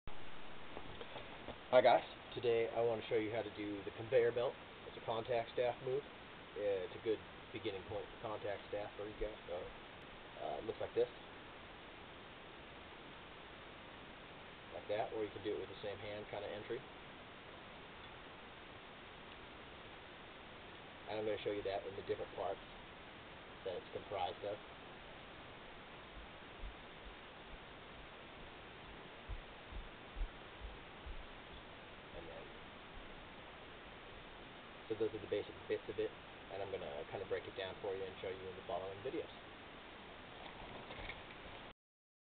Basic Contact Staff Lesson - Conveyor Belt Overview (AKA Elbow/Wrist/Wrist/Elbow)
A quick overview of what I'm going to teach in this short video series.  It's a combo of simple contact staff moves that make one really cool move.  I hope the videos help you learn 'em, 'cause they're really fun!